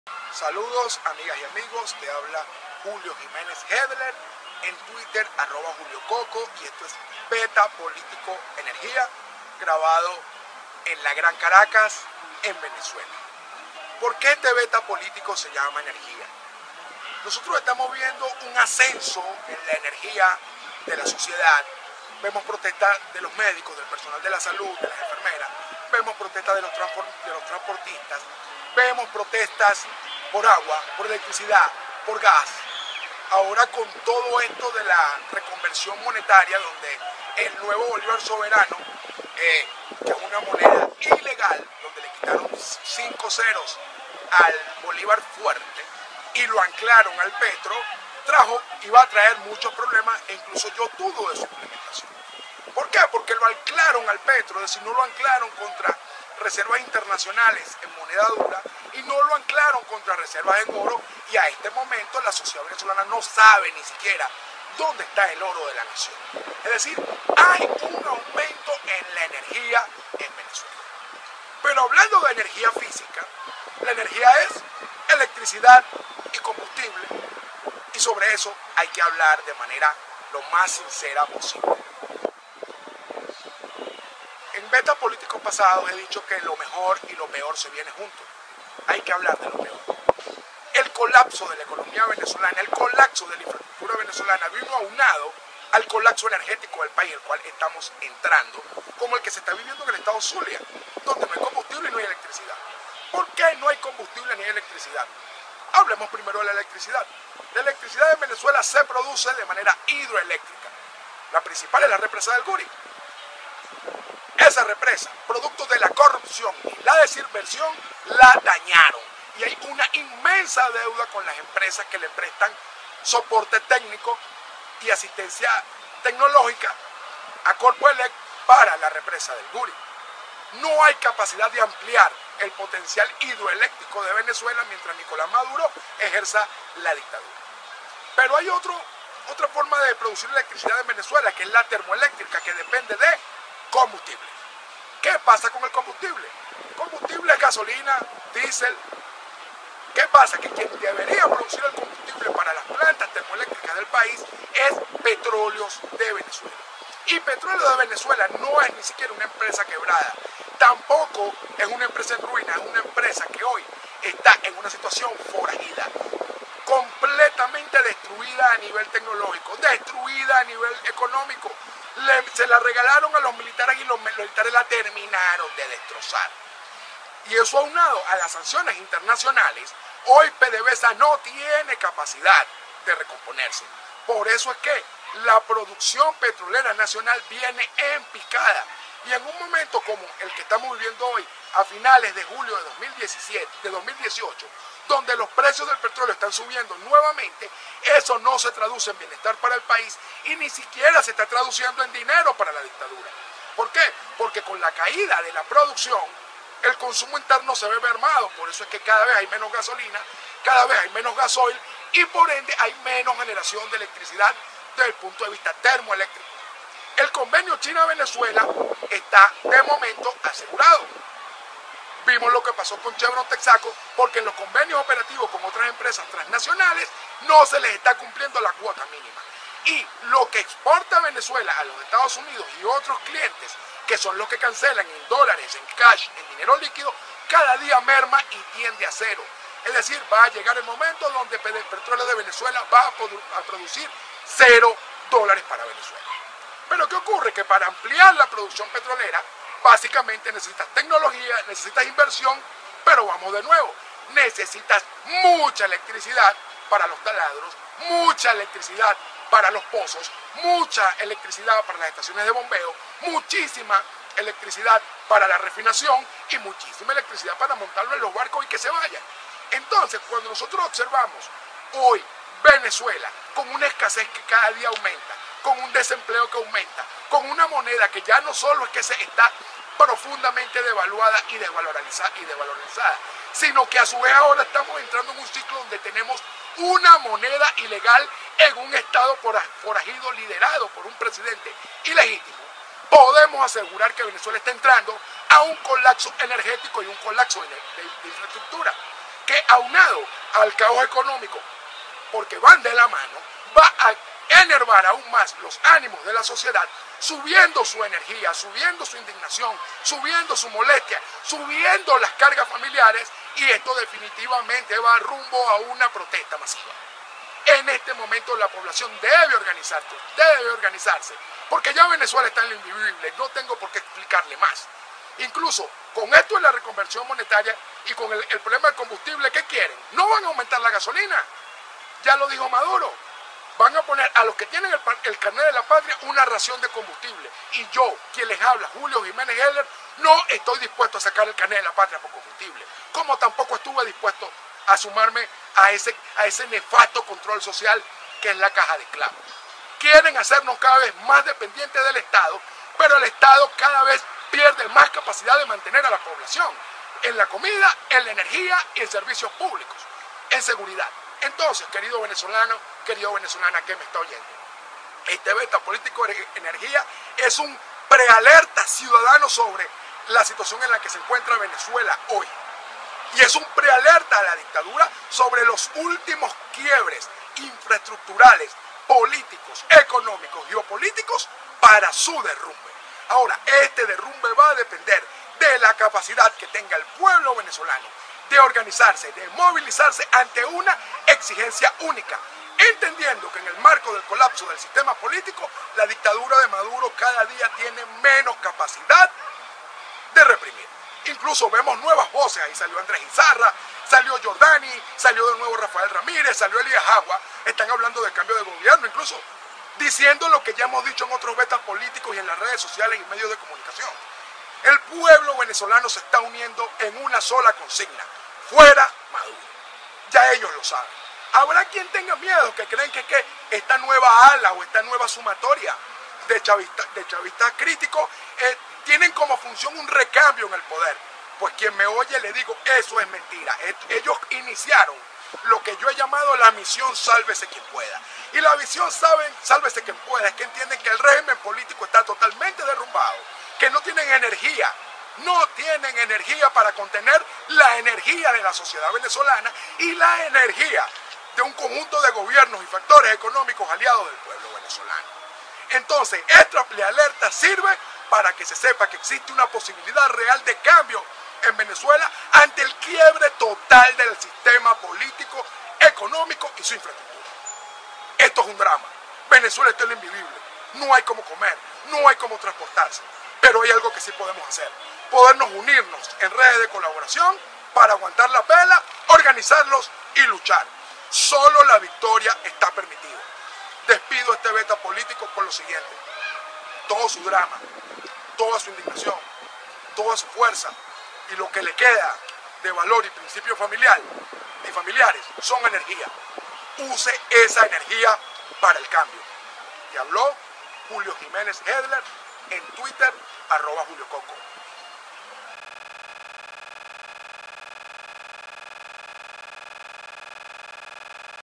BetaPolítico Energía 30Jul
Análisis y propuestas sobre la situación política en Venezuela y perspectivas para el cambio necesario.